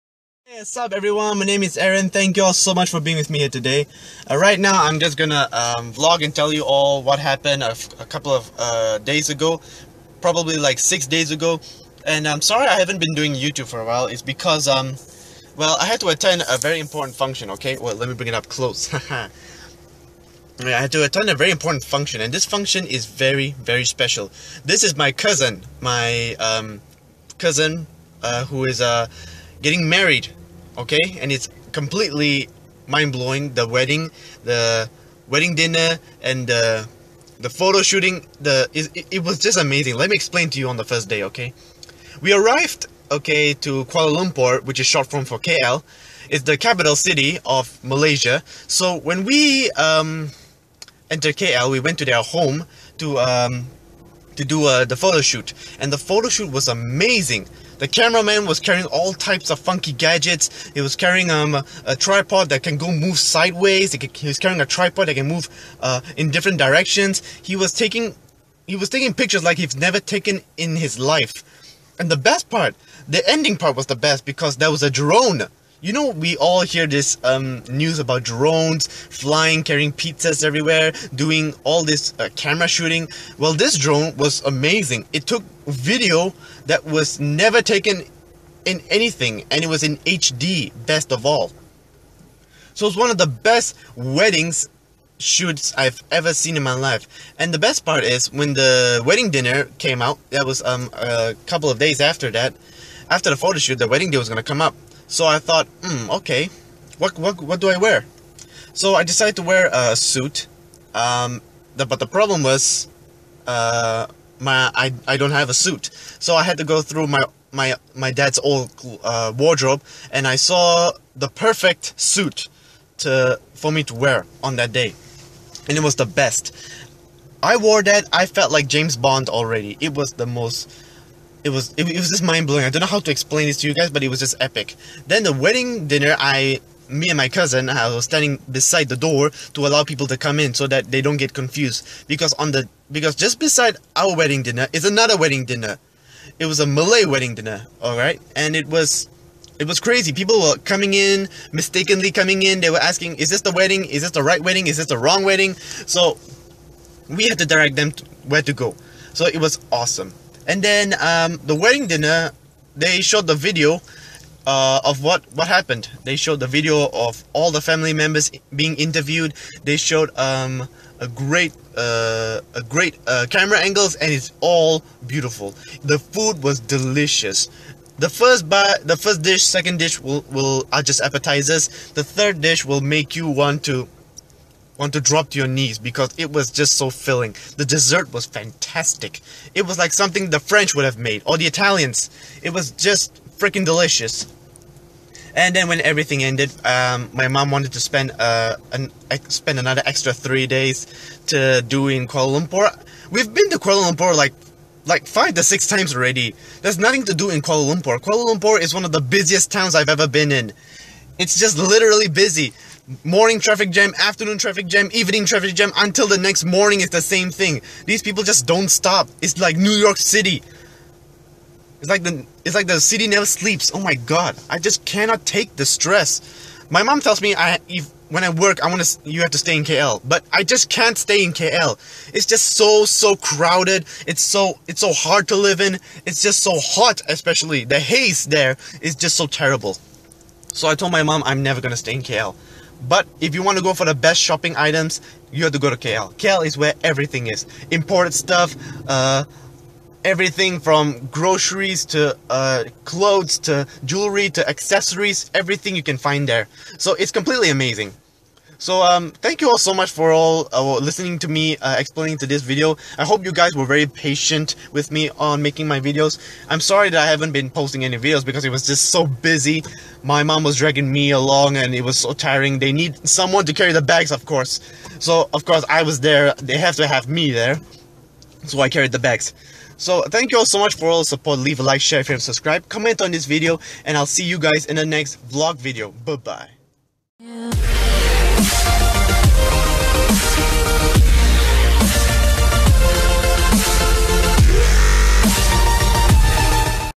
Missing In Action - Aaron JC Vlogs
Hi everyone, I'm please to tell you all that I am back from my trip to Kuala Lumpur (KL) and now I will be continuing making new and awesome videos for you guys. Thank you for all your support and for your patience. I'll keep cranking out more amazing videos for you guys.

I love you all for supporting me and for watching my videos, happy to know that there are people who appreciate.